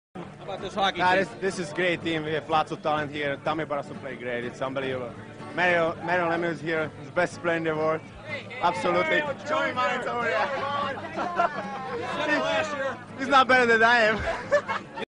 Young Jaromir Jagr after winning Stanley Cup 1992
great memories